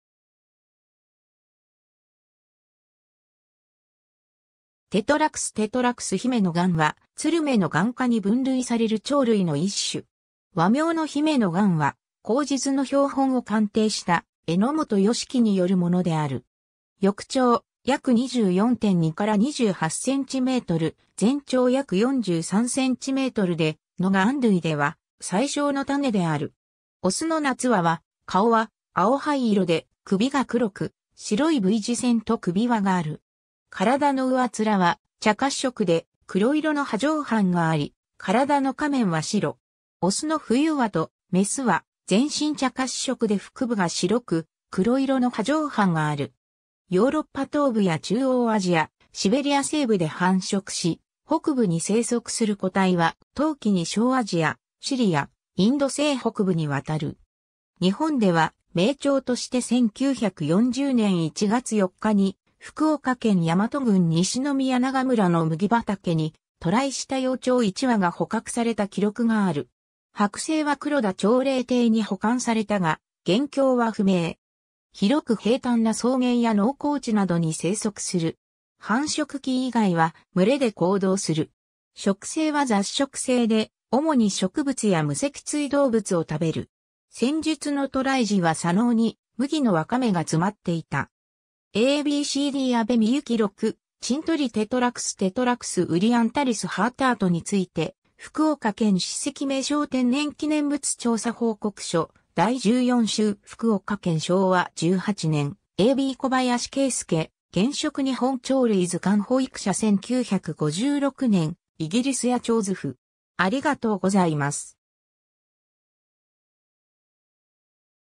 ヒメノガン
Tetrax tetrax ヒメノガン (姫野雁、学名：Tetrax tetrax)は、ツル目ノガン科に分類される鳥類の一種。
和名のヒメノガンは、後述の標本を鑑定した榎本佳樹によるものである。
 翼長約24.2~28cm、全長約43cmで、ノガン類では最小の種である。
雄の夏羽は、顔は青灰色で頸が黒く、白いV字線と首輪がある。
体の上面は茶褐色で黒色の波状斑があり、体の下面は白。
雄の冬羽と雌は全身茶褐色で腹部が白く、黒色の波状斑がある。
 ヨーロッパ東部や中央アジア、シベリア西部で繁殖し、北部に生息する個体は冬期に小アジア、シリア、インド西北部に渡る。
日本では迷鳥として1940年（昭和15年）1月4日に福岡県山門郡西宮永村（現在の福岡県柳川市）の麦畑に渡来した幼鳥1羽が捕獲された記録がある。
剥製は黒田長礼邸に保管されたが、現況は不明。
 広く平坦な草原や農耕地などに生息する。
繁殖期以外は群れで行動する。
 食性は雑食性で、おもに植物や無脊椎動物を食べる。
先述の渡来時は砂嚢に麦の若芽が詰まっていた。
 ^ a b c d 安部幸六「珍鳥Tetrax tetrax orientalis HARTERTに就て」 『福岡縣史蹟名勝天然紀念物調査報告書 第十四集』福岡縣 昭和18年 ^ a b 小林桂助『原色日本鳥類図鑑』 保育社 1956年 イギリス野鳥図譜